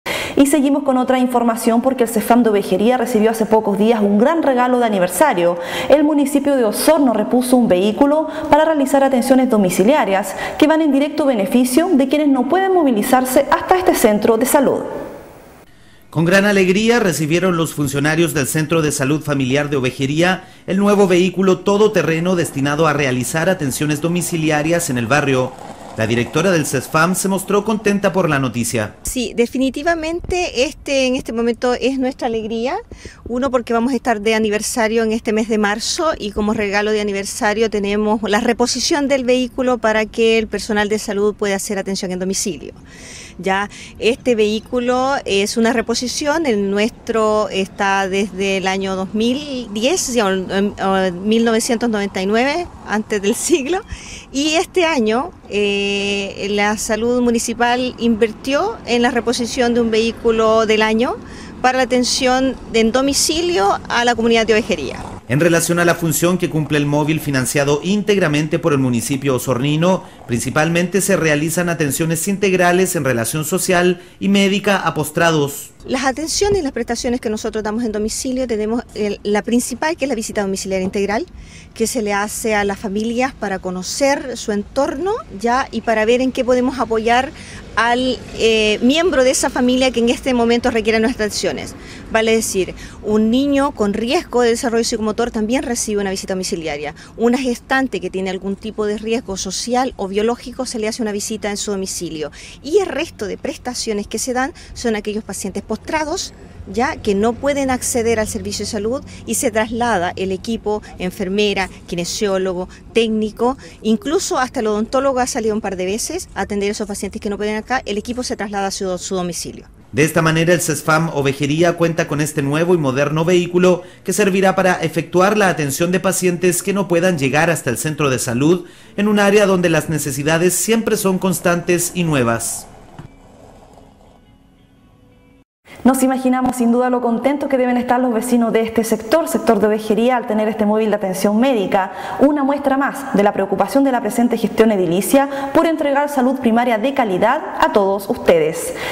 Nuevo vehiculo para CESFAM Ovejeria.
CESFAM Ovejeria recibio nuevo vehiculo para atenciones domiciliarias.